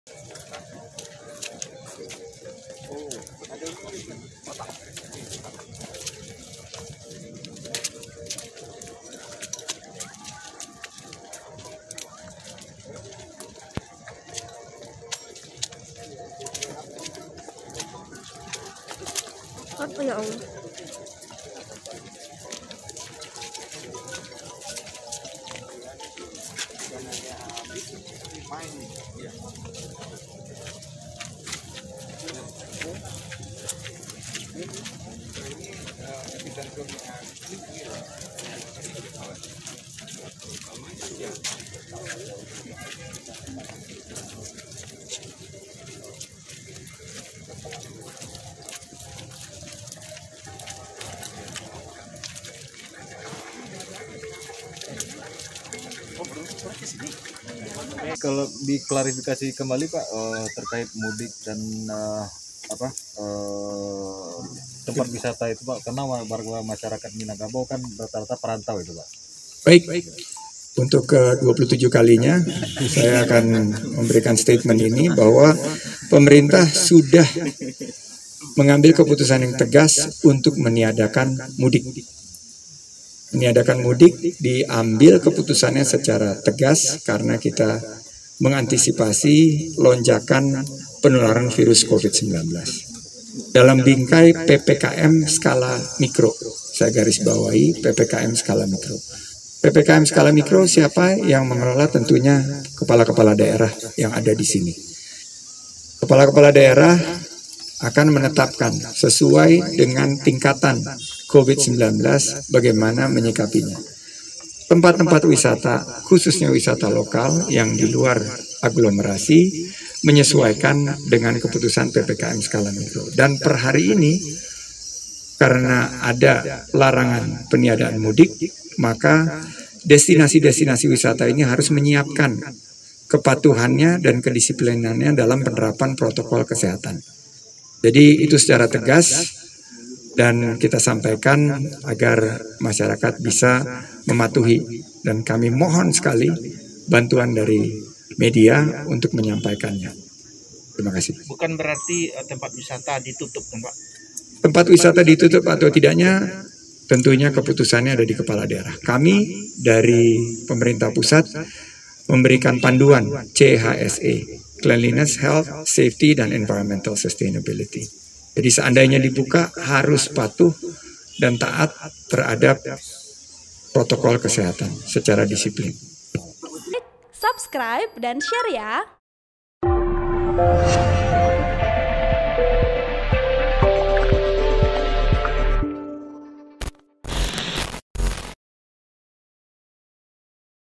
Buka atau Tutup Objek Wisata saat Mudik Lebaran, Sandiaga Uno: Keputusan Ada di Kepala Daerah
TRIBUN-VIDEO.COM - Menteri Pariwisata dan Ekonomi Kreatif (Menparekraf) Republik Indonesia, Sandiaga Salahuddin Uno mengatakan pemerintah sudah mengambil keputusan yang tegas untuk meniadakan mudik.

Menurutnya, memang larangan mudik menjadi suatu keprihatian dari semua pihak.

Akan tetapi keputusan larangan mudik diambil dengan berbagai macam pertimbangan.

"Kebijakan tidak mudik itu sudah final. Sudah dilakukan secara nasional," terang Sandiaga.

Selain itu, menurutnya, keputusan tersebut diambil secara tegas dalam rangka mengantisipasi lonjakan penularan virus Covid-19 dalam bingkai PPKM skala mikro.

"PPKM skala mikro yang mengelola tentunya kepala daerah. Kepala daerah akan menetapkan sesuai dengan tingkatan Covid -19 bagaimana menyikapinya," tutur Sandiaga Uno.

Sandiaga Uno menambahkan, karena sudah ada larangan mudik, maka destinasi wisata harus menyiapkan kepatuhan dan kedisiplinannya dalam penerapan protokol kesehatan.

Dia berharap masyarakat bisa mematuhi hal itu sehingga angka penyebaran Covid-19 dapat ditekan.

"Tempat wisata ditutup atau tidaknya, tentunya keputusan ada di kepala daerah. Kami dari pemerintah pusat memberikan panduan CHSE, seandainya dibuka harus patuh dan taat terhadap protokol kesehatan secara disiplin," terang Sandiaga Uno.(*)